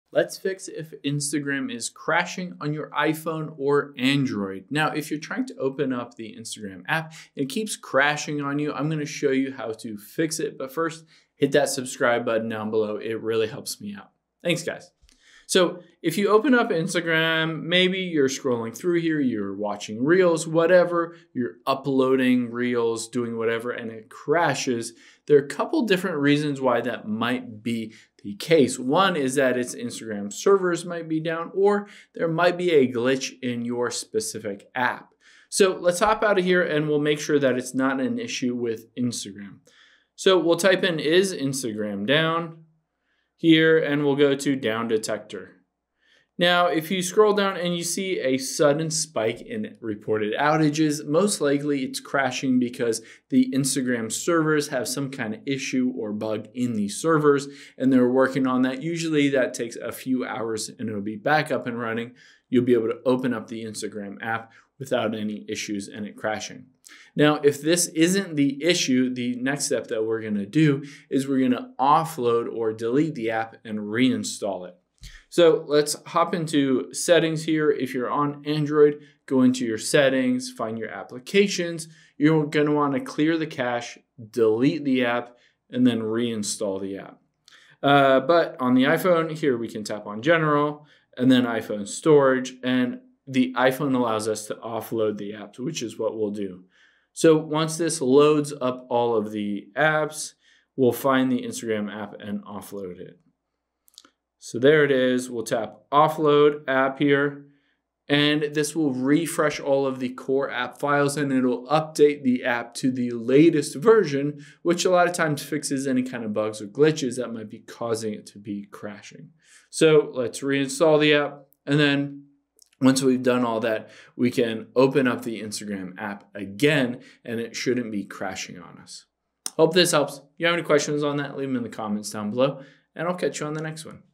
How To Fix Instagram Crashing On iPhone Or Android
Let's fix the Instagram app if it keeps crashing every time you open it on your iPhone or Android.